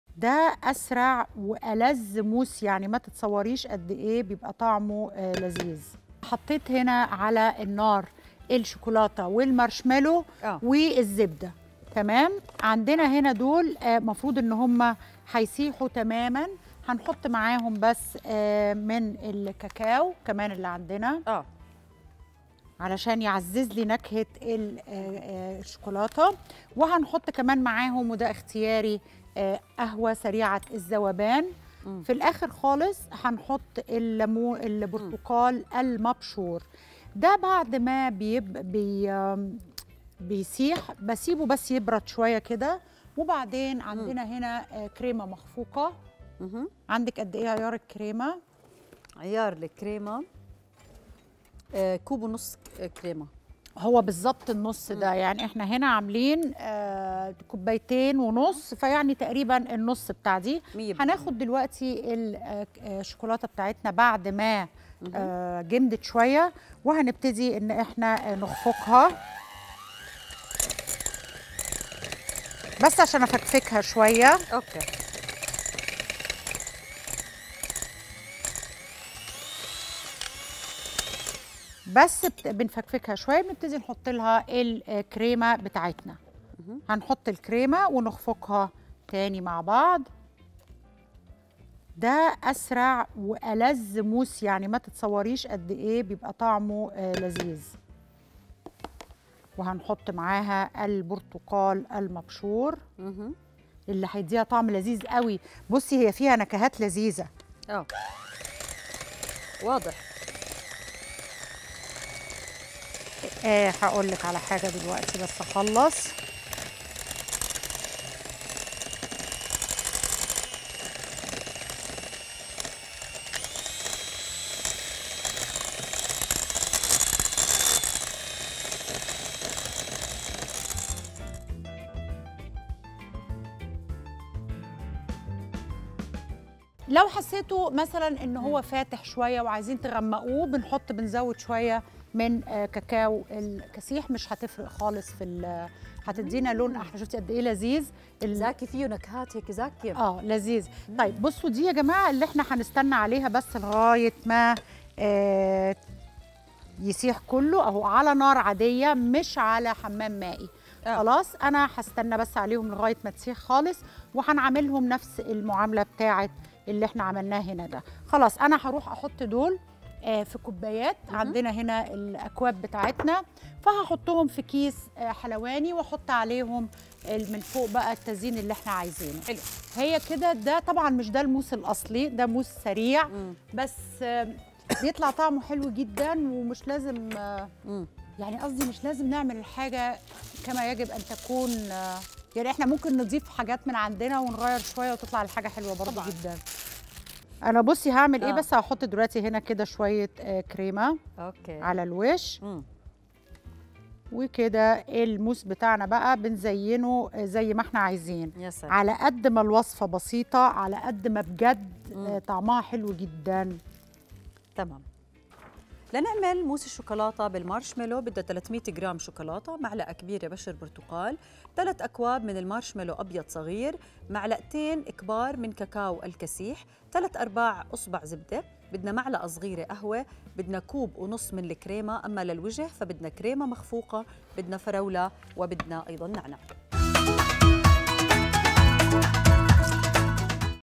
أسرع وألز موس شكولاته بالعالم
المقادير :

٣٠٠ جم شوكولاته
١ ملعقة كبيرة بشر برتقال
٣ كوب مارشميلو أبيض صغير
٢ ملعقة كبيرة كاكاو الكسيح
٣/٤ اصبع زبده
١ ملعقة صغيرة قهوة
١ ١/٢ كوب كريمة
للوجه
كريمة مخفوقة
فراولة
نعناع

مطبخ رؤيا